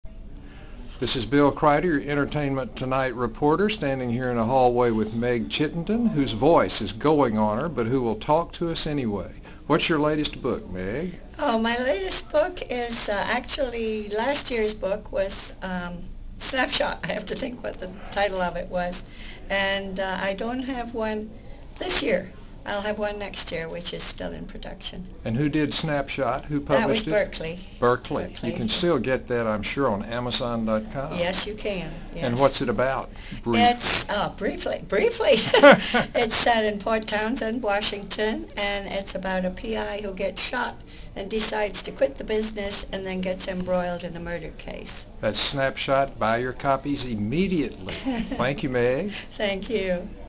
Meg Chittenden at Bouchercon
Meg talks about her books.